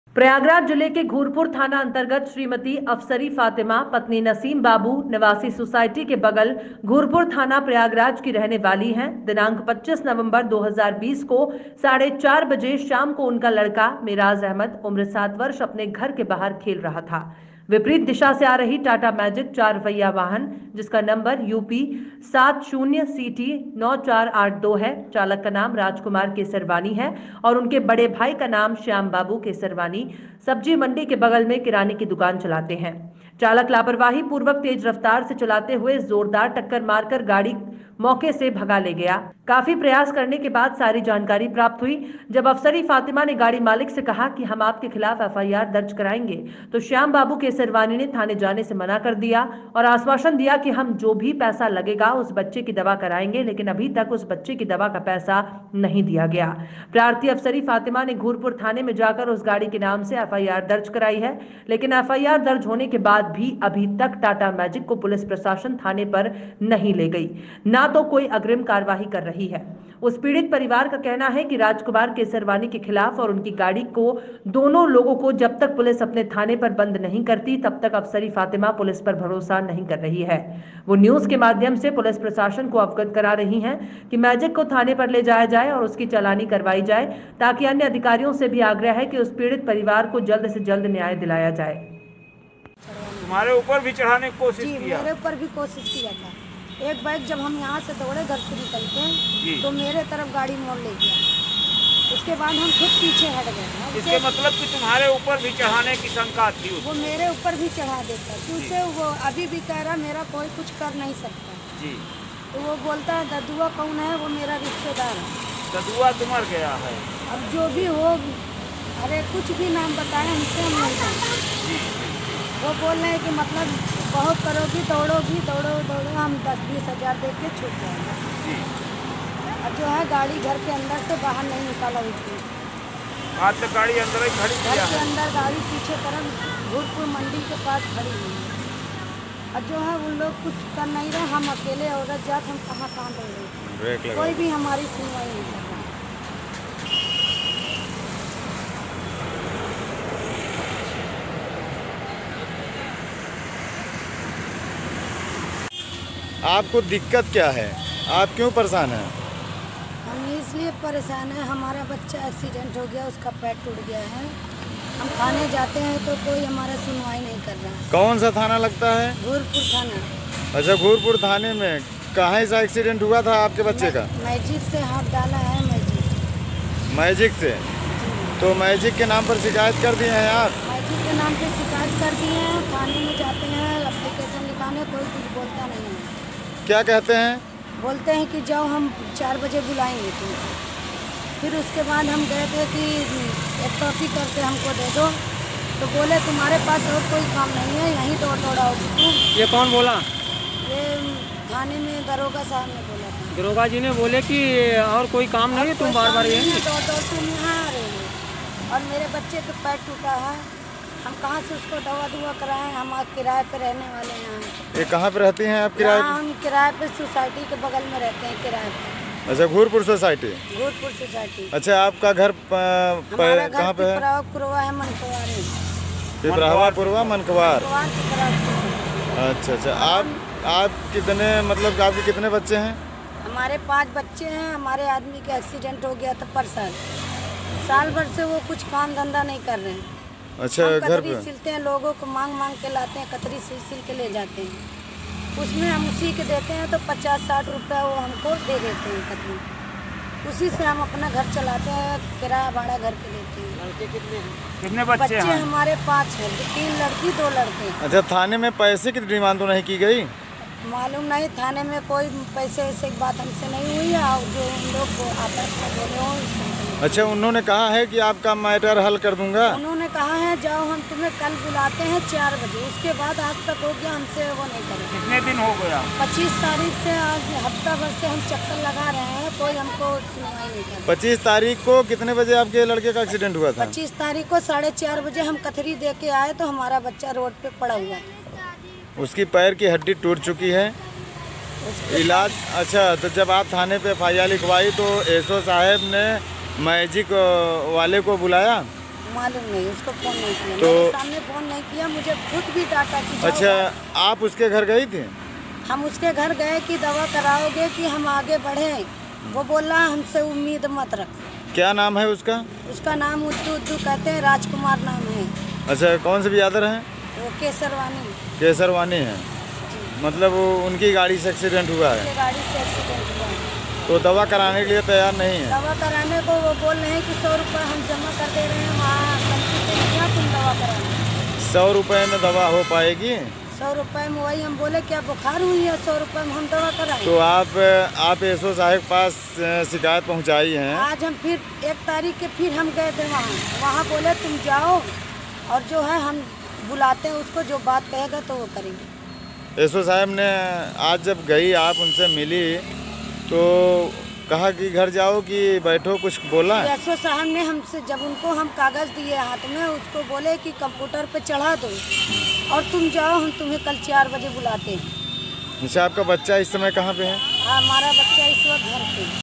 घूरपुर पुलिस द्वारा मुकदमा दर्ज होने के बावजूद भी टाटा मैजिक को नहीं किया गया गिरफ्तार
प्रयागराज जिले के घूरपुर थाना अंतर्गत श्रीमती अफसरी फातिमा पत्नी नसीम बाबू निवासी- सोसाइटी के बगल घूरपुर थाना प्रयागराज की रहने वाली हैं दिनांक 25 /11 /2020 को 4:30 बजे शाम को उनका लड़का मेराज अहमद उम्र 7 वर्ष अपने घर के बाहर खेल रहा था कि विपरीत दिशा से आ रही टाटा मैजिक चार पहिया वाहन जिसका नंबर UP 70 CT 9482 है !चालक का नाम राजकुमार केसरवानी और उनके बड़े भाई का नाम श्याम बाबू केसरवानी सब्जी मंडी के बगल में किराने की दुकान चलाते हैं !चालक लापरवाही पूर्वक तेज रफ्तार से चलाते हुए जोरदार टक्कर मार कर गाड़ी को मौके से भगा ले गया काफी प्रयास करने के बाद सारी जानकारी प्राप्त हुई जब अफसरी फातिमा ने गाड़ी मालिक से कहा कि हम आप के खिलाफ FIR दर्ज कराएंगे तो श्याम बाबू केसरवानी ने थाने जाने से मना कर दिया और आश्वासन दिया कि हम जो भी पैसा लगेगा उस बच्चे की दवा कराएंगे लेकिन अभी तक उस बच्चे की दवा का पैसा नहीं दिया गया प्रार्थी अफसरी फातिमा ने घूरपुर थाने में जाकर उस गाड़ी के नाम से FIR दर्ज कराया !लेकिन FIR दर्ज होने के बाद भी अभी तक टाटा मैजिक को पुलिस प्रशासन थाने पर नहीं ले गई ना तो कोई अग्रिम कार्रवाई कर रही है उस पीड़ित परिवार का कहना है! राजकुमार केसरवानी के खिलाफ और उनकी गाड़ी को दोनों लोगों को जब तक पुलिस अपने थाने पर बंद नहीं करती तब तक अफसरी फातिमा पुलिस पर भरोसा नहीं कर रही हैं वह न्यूज़ के माध्यम से पुलिस प्रशासन को अवगत करा रही हैं कि मैजिक को थाने पर ले जाया जाए और उसकी चालानी कार्रवाई की जाए ताकि अन्य अधिकारियों से भी आग्रह है कि उस पीड़ित परिवार को जल्द से जल्द न्याय दिलाया जाए!